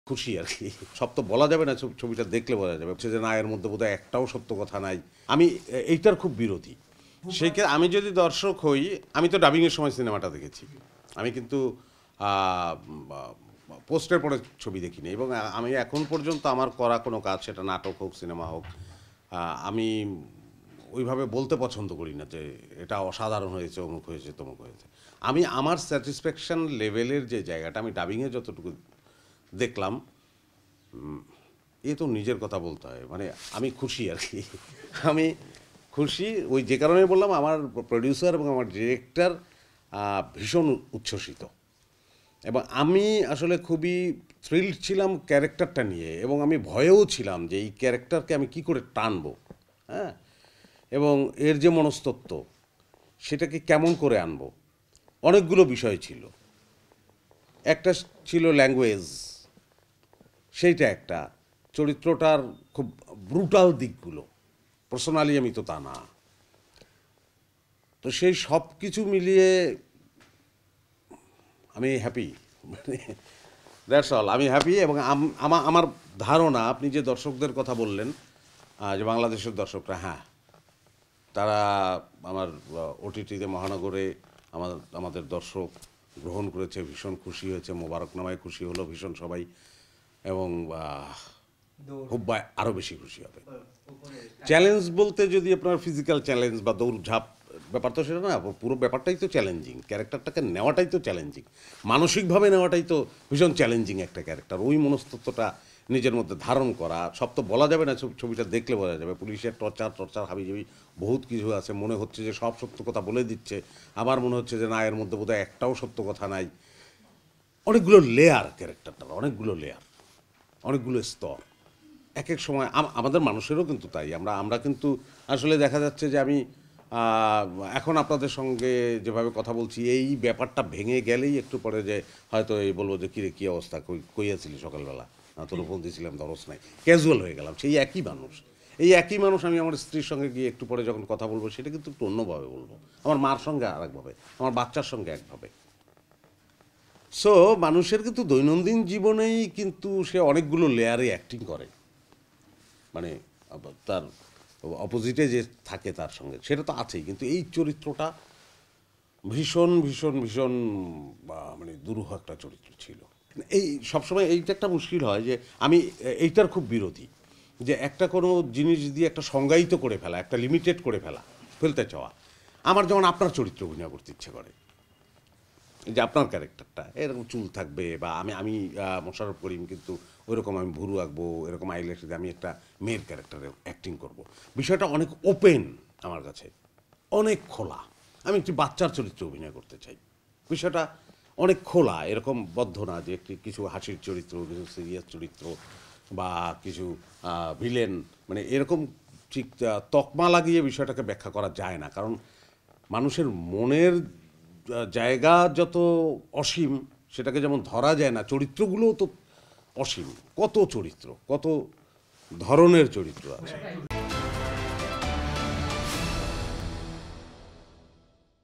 বড় চুলের মেয়েদের চরিত্রে অভিনয় করতে চান মোশররফ করিম | Mosharraf Karim | Desh TV
42, Shaheed Sangbadik Selina Parveen Sarak, Malibag 1217 Dhaka, Dhaka Division, Bangladesh